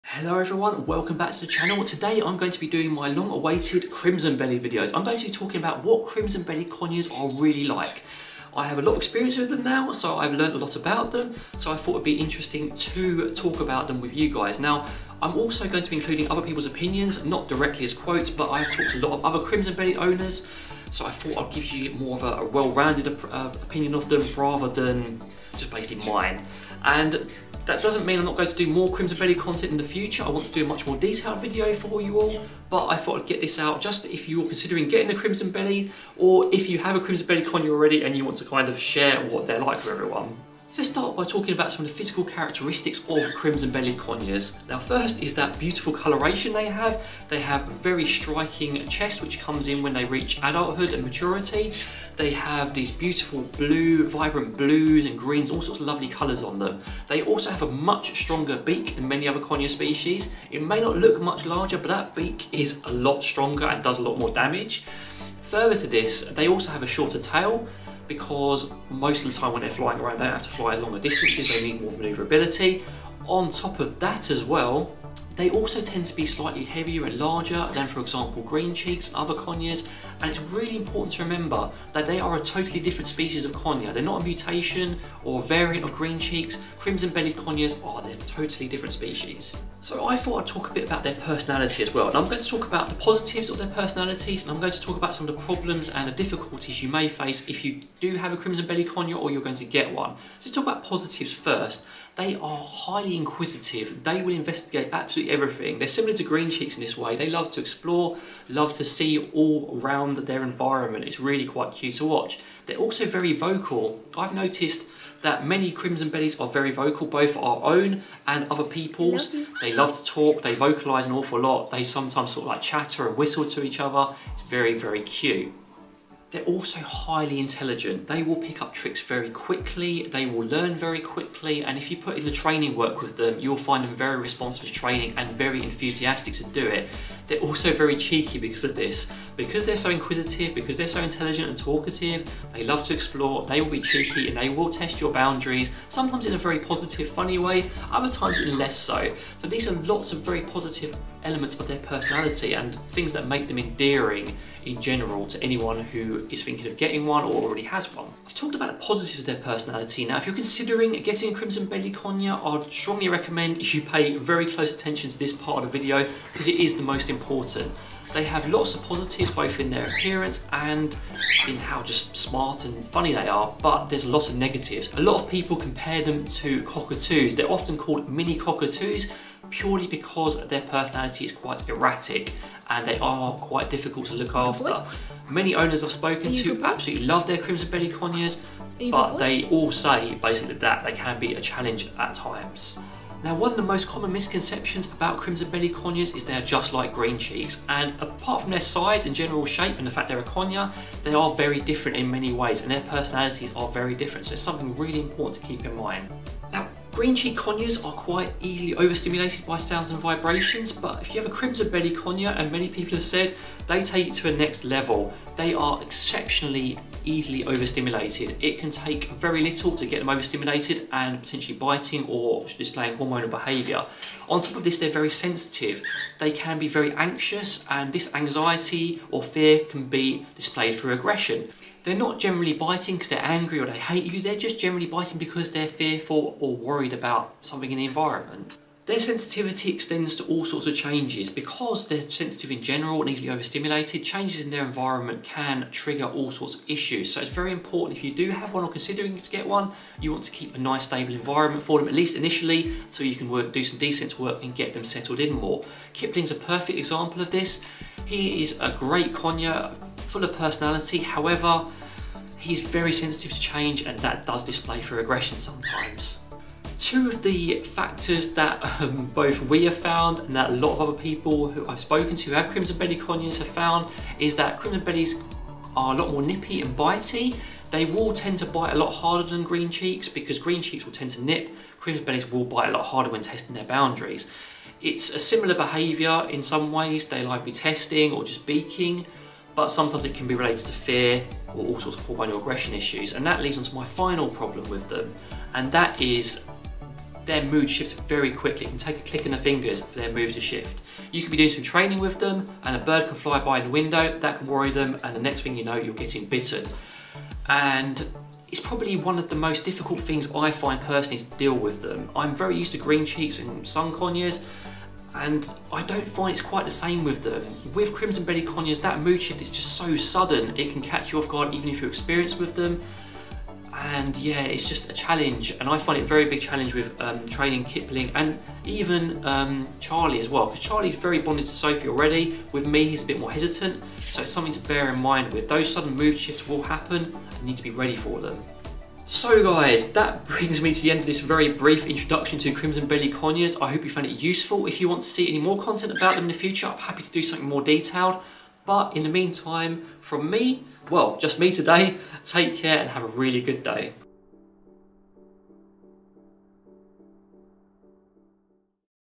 What Crimson Bellied Conures Are REALLY Like! | TheParrotTeacher
Hey conure lovers!

The long awaited video on crimson bellied conures. I feel there isn't enough information out there about them so I thought I'd start by making a general video about their physical traits, personality and behaviour.

Don't forget, every parrot is an individual so you will see different behaviours in each parrot depending on so many things.

But the information presented in this video is not just from my experiences of Crimson bellies, but from many other owners experiences across various platforms.

Hope you find it useful and if you want more crimson content do let me know!

🛒 You can get lots of parrot goodies from our friends over at Northern Parrots *